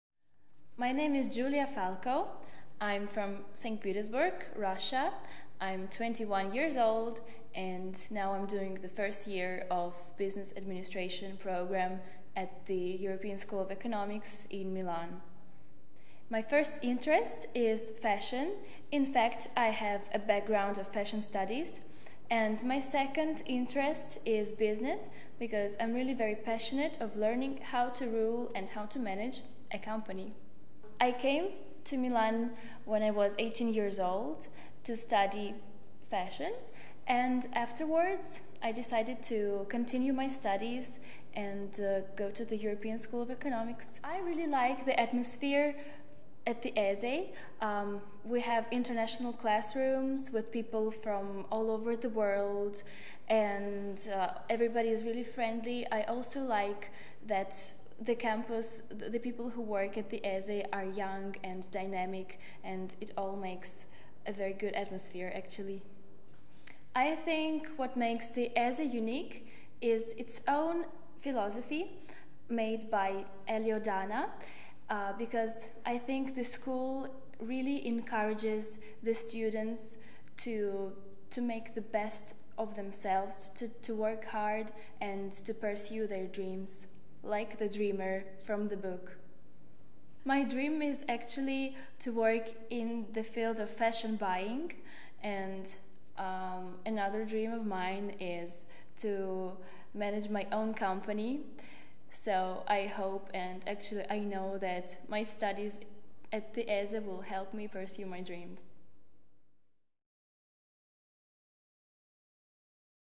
European School of Economics - Student's Review by Julia Falko
Bsc in Business Administration student gives hes opinion of the European School of Economics Milan.

Location: Superstudio via Forcella - ESE Milan Centre.
Opinions, reviews and testimonies from students.
Opinioni, pareri e testimonianze delle studenti ESE.